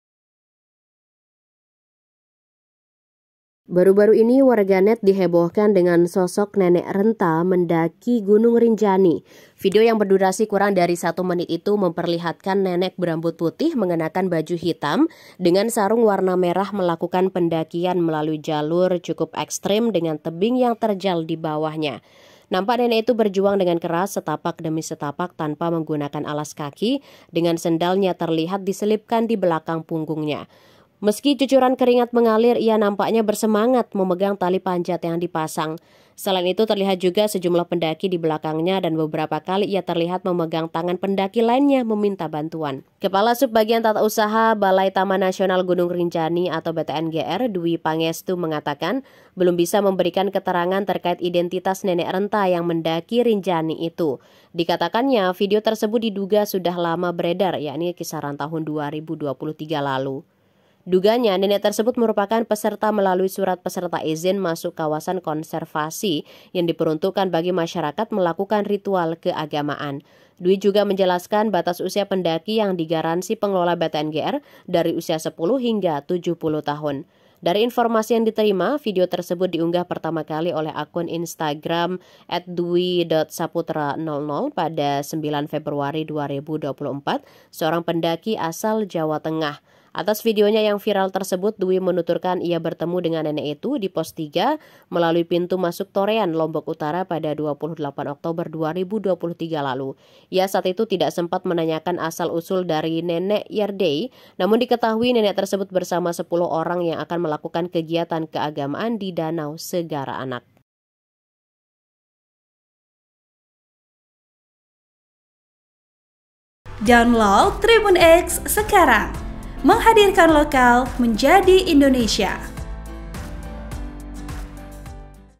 VIRAL Video Nenek Mendaki Gunung Rinjani, Penjelasan BTNGR: Naik untuk Ritual Keagamaan
Laporan Wartawan TribunLombok.com, Ahmad Wawan Sugandika

TRIBUNLOMBOK.COM, LOMBOK TIMUR - Baru-baru ini warganet dihebohkan dengan sosok nenek renta mendaki Gunung Rinjani.

Video yang berdurasi kurang dari satu menit itu memperlihatkan nenek berambut putih mengenakan baju hitam dengan sarung warna merah melakukan pendakian melalui jalur cukup ekstrem dengan tebing yang terjal di bawahnya.

Nampak nenek itu berjuang dengan keras, setapak demi setapak tanpa menggunakan alas kaki, dengan sendalnya terlihat diselipkan di belakang punggungnya.

Meski cucuran keringat mengalir, ia nampak bersemangat, memegang tali panjat yang dipasang.

Selain itu, terlihat juga sejumlah pendaki di belakangnya, dan beberapa kali ia terlihat memegang tangan pendaki lainnya meminta bantuan.

Penjelasan BTNGR

Kepala Sub Bagian Tata Usaha, Balai Taman Nasional Gunung Rinjani (BTNGR), Dwi Pangestu mengatakan belum bisa memberikan keterangan terkait identitas nenek renta yang mendaki Rinjani itu.

Dikatakannya, video tersebut diduga sudah lama beredar, yakni kisaran tahun 2023 yang lalu.

"Ini udah lama, tahun 2023, kami belum bisa mendeteksi identitas yang ada di video butuh waktu," ucap Dwi setelah dikonfirmasi, Selasa (12/3/2024).

Dugaannya, nenek tersebut merupakan peserta melalui surat peserta izin masuk kawasan konservasi (Simaksi) yang diperuntukkan bagi masyarakat melakukan ritual keagamaan.

"Iya ada izin khusus memang, untuk kegiatan pendidikan penelitian dan budaya seperti pendaki ritual masuk di dalamnya," kata Dwi.

Dwi juga menjelaskan batas usia pendaki yang digaransi pengelola BTNGR dari usia 10 hingga 70 tahun.

"Setiap pendaki wajib pakai asuransi, dikebijakan asuransi yang di-cover usia 10 sampai 70 tahun," demikian Dwi.

Dari informasi yang diterima, video tersebut diunggah pertama kali oleh akun Instagram @dwi.saputra00, pada 9 Februari 2024, seorang pendaki asal Jawa Tengah.

Atas videonya yang viral tersebut, Dwi menuturkan ia bertemu dengan nenek itu di pos 3 melalui pintu masuk Torean, Lombok Utara, pada 28 Oktober 2023 lalu.

"Iya saya terkejut ada nenek tua mampu naik Rinjani, dengan kondisi seperti itu dia masih bersemangat," kata Dwi Saputra.

Ia saat itu tidak sempat menanyakan asal usul dari nenek yerdei, namun diketahui nenek tersebut bersama 10 orang yang akan melakukan kegiatan keagamaan di Danau Segara Anak.

"Informasi yang saya dapatkan dulu itu, katanya ada kegiatan umat Hindu di Segara Anak," demikian Saputra.

Sumber: Tribun Lombok

Artikel ini telah tayang di TribunLombok.com dengan judul VIRAL Video Nenek Mendaki Gunung Rinjani, Penjelasan BTNGR: Naik untuk Ritual Keagamaan,
Penulis: Ahmad Wawan Sugandika | Editor: Endra Kurniawan